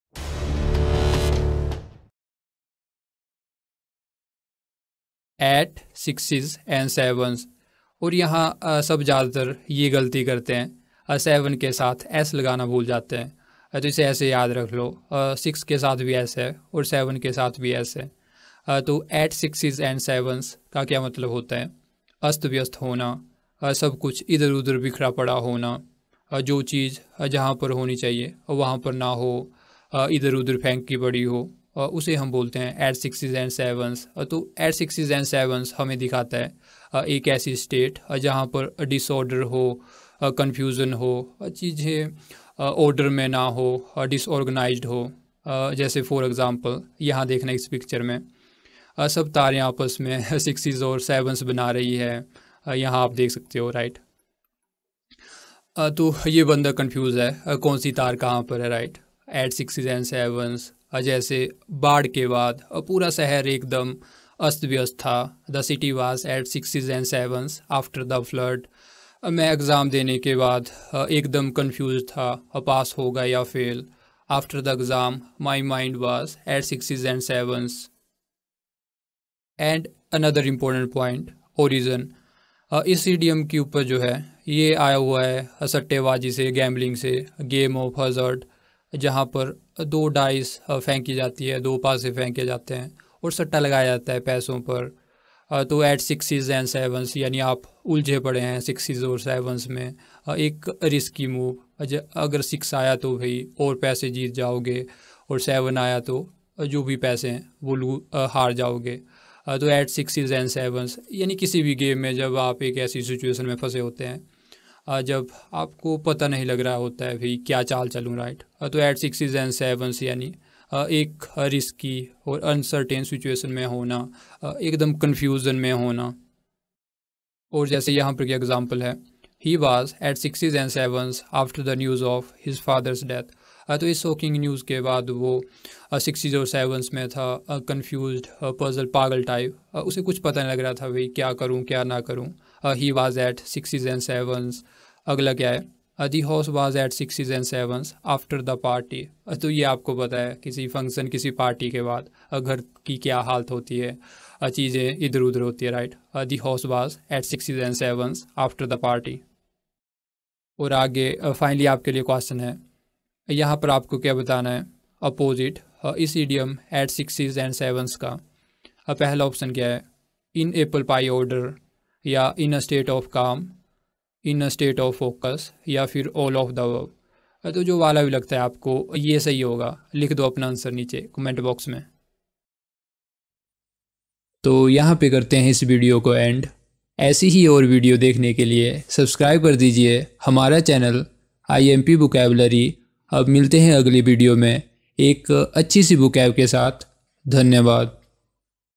at sixes and sevens idiom phrase trick origin story sentence MCQ example Meaning in Hindi Imp Vocab
idiom meaning in Urdu with examples  | Imp Vocabulary | by Rahul Thakur

At sixes and sevens
Meaning
= to be in a state of confusion or disorder

Example
= He was at sixes and sevens after the news of his father's death.
= The house was at sixes and sevens after the party.